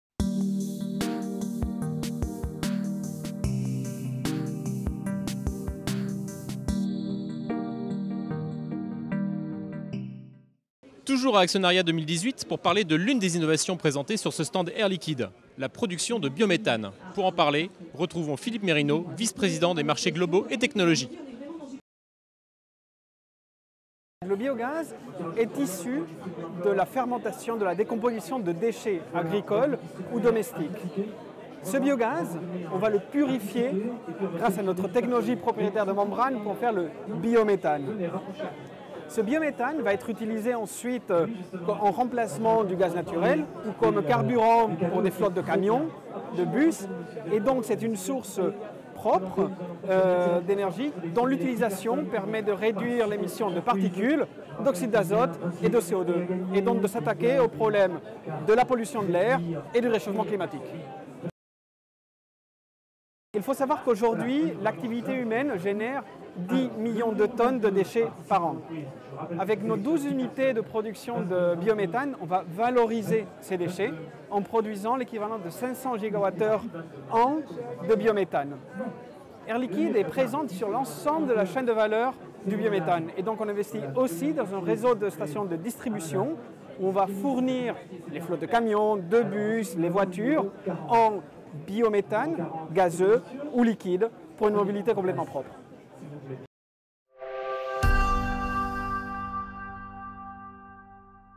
Air Liquide @ Actionaria 2018 - Focus sur le biométhane
Entretien avec Philippe Merino, Directeur Marchés avancés et technologies d'Air Liquide.